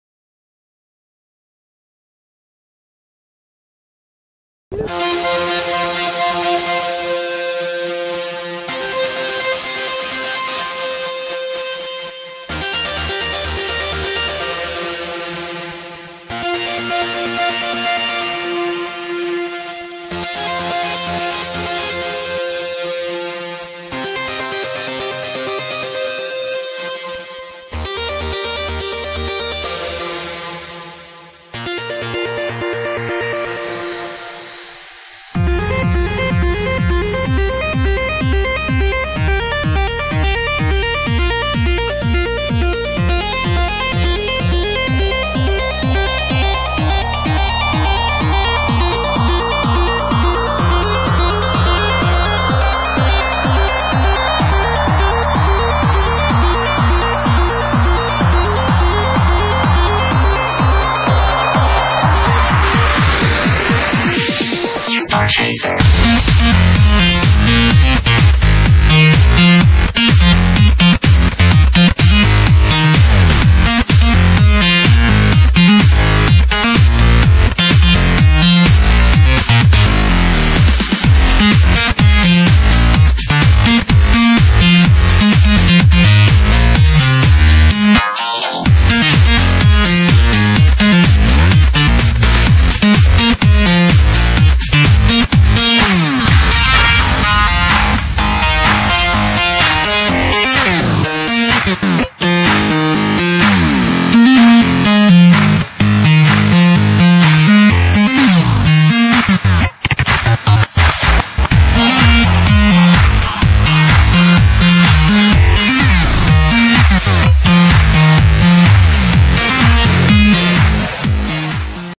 Gopro HD - waterslide / Québec city
waterslides with gopro at village vacance valcartier in québec, Canada... enjoy